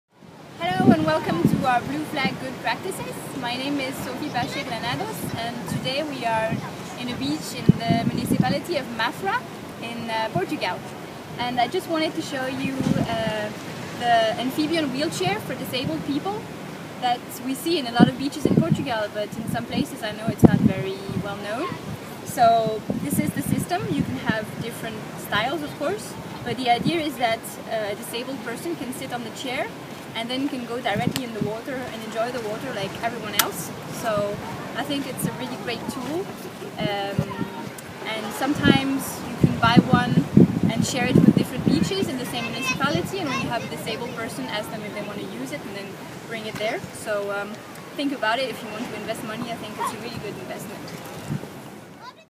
Amphibian Wheelchair For Disabled People
A great service for disabled people as with this wheelchair they can go in the water.